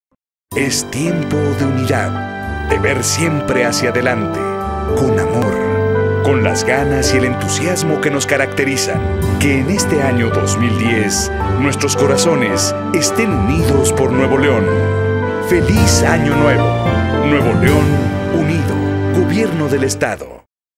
Mensaje de año nuevo del Gobierno de Nuevo León | 2009
Es tiempo de unidad, de ver siempre hacia delante, con amor, con las ganas y el entusiasmo que nos caracterizan. Que en este Año 2010 nuestros corazones estén unidos por Nuevo León. Feliz Año Nuevo.